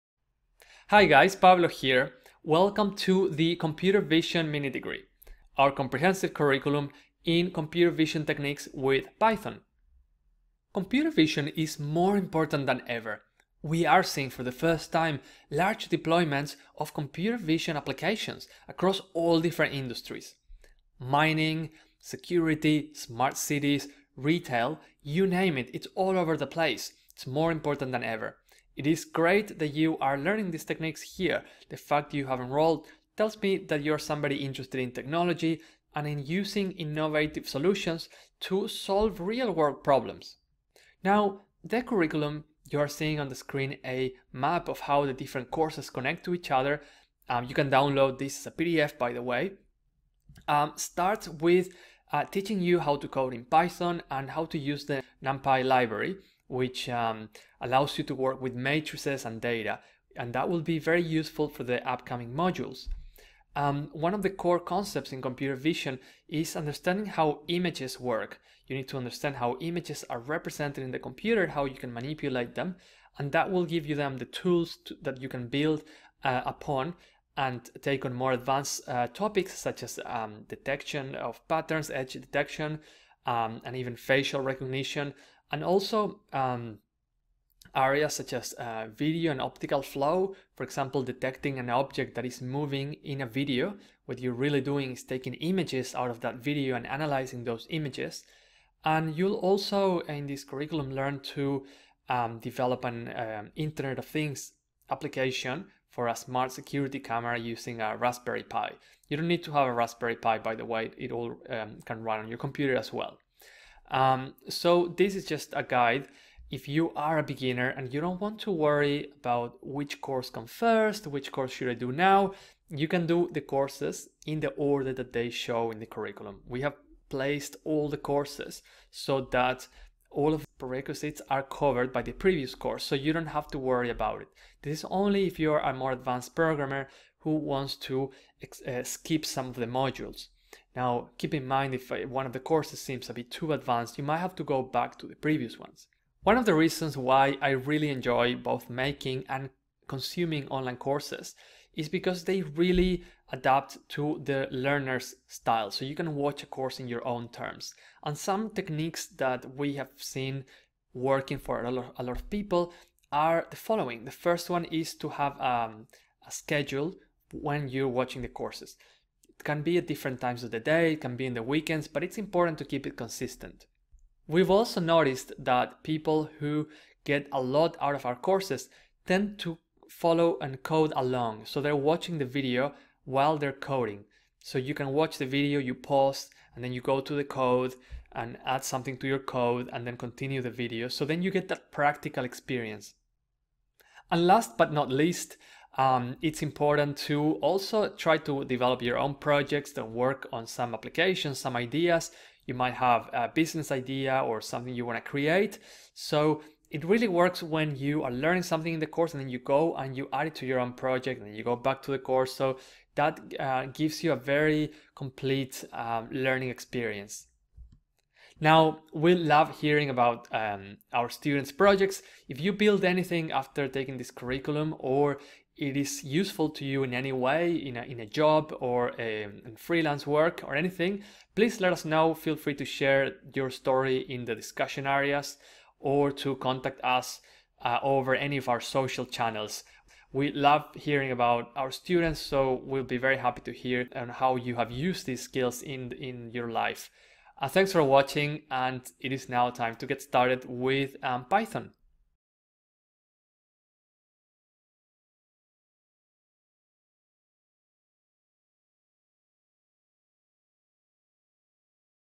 Introduction to the Python Computer Vision Mini Degree
The Python Computer Vision Mini-Degree is an on-demand curriculum featuring 8 professional-grade courses that are suitable for beginners and more advanced developers alike. This innovative program will teach you how to code in Python, master the fundamentals of Numpy, and understand the core concepts underlying computer vision, including how images are represented and can be manipulated, pattern detection, and video and optical flow.

With such an understanding of these powerful technologies, you’ll be prepared to take on the world as a well-informed and highly prepared developer!